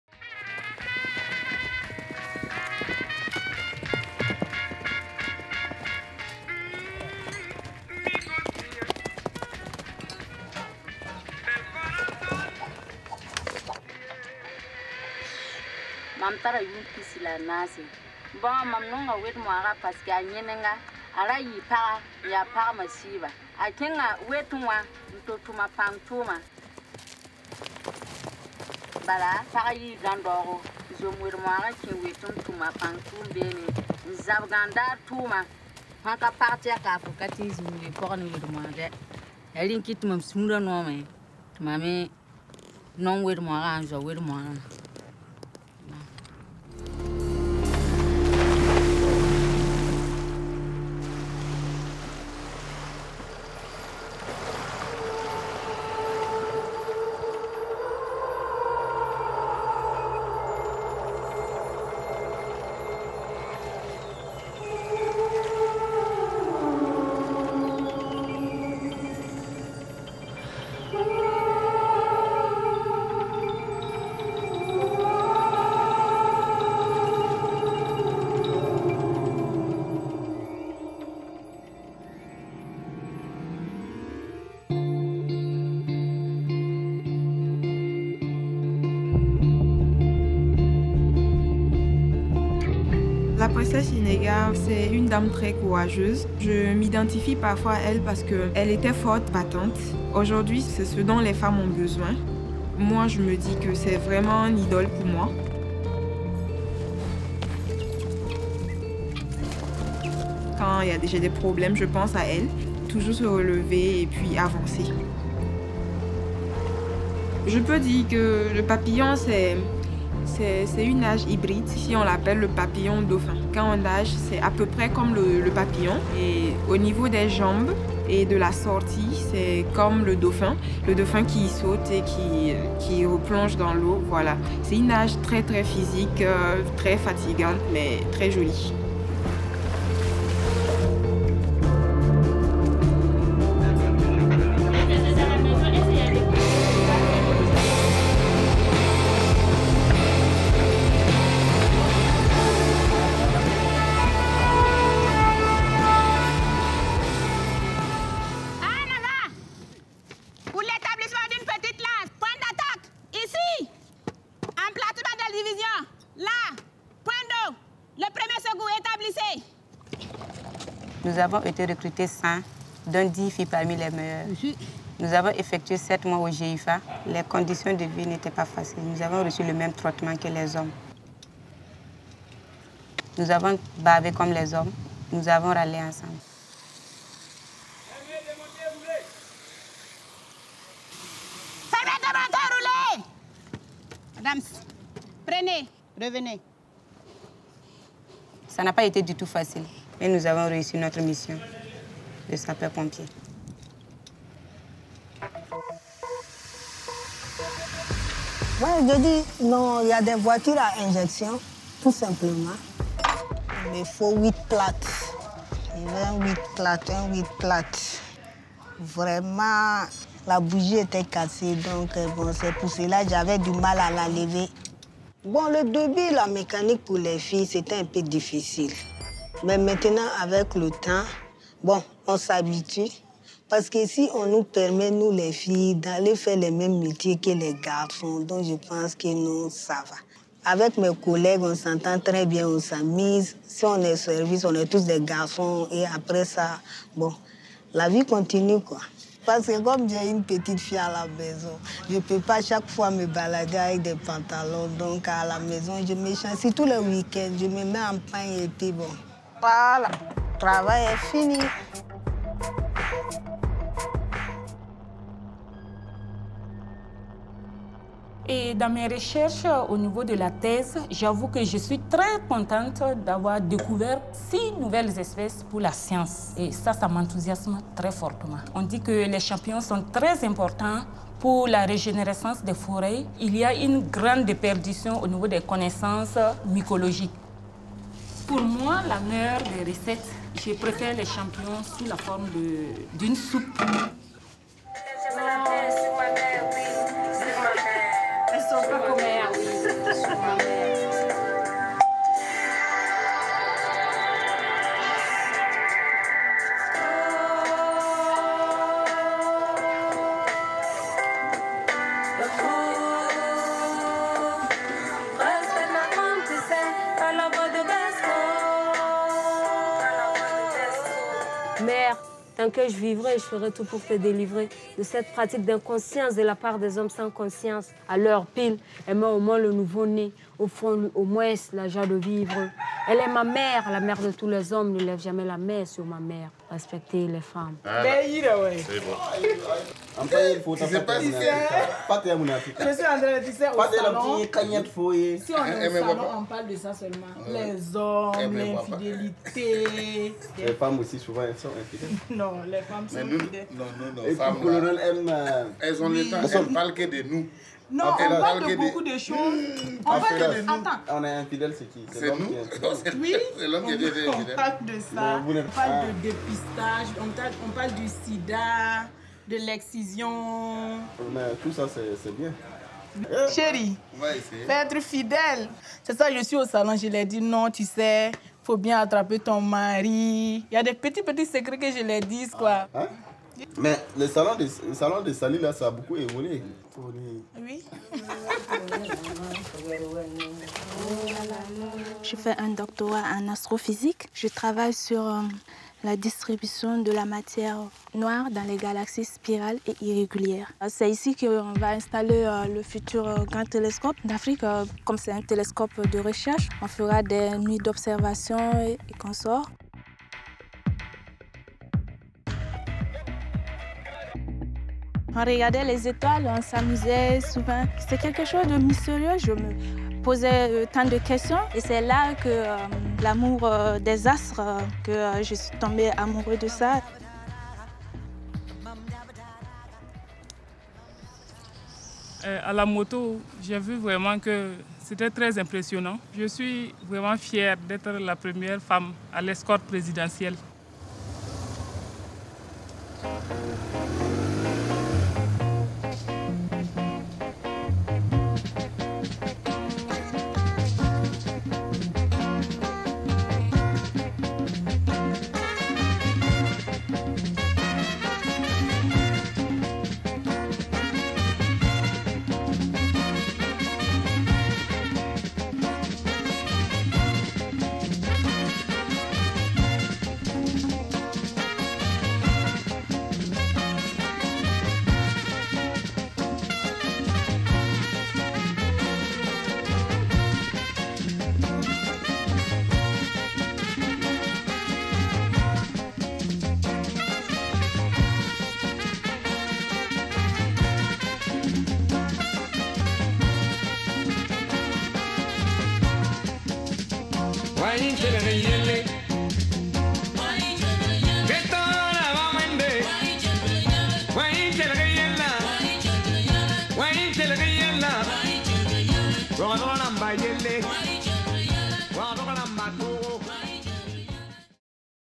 U2 filmmaker Nicole Mackinlay Hahn meets the women of Burkina Faso
Meet the women of Burkina Faso who are channeling the fighting spirit of a 12th-century warrior princess. From U2 filmmaker Nicole Mackinlay Hahn. Read the full feature on NOWNESS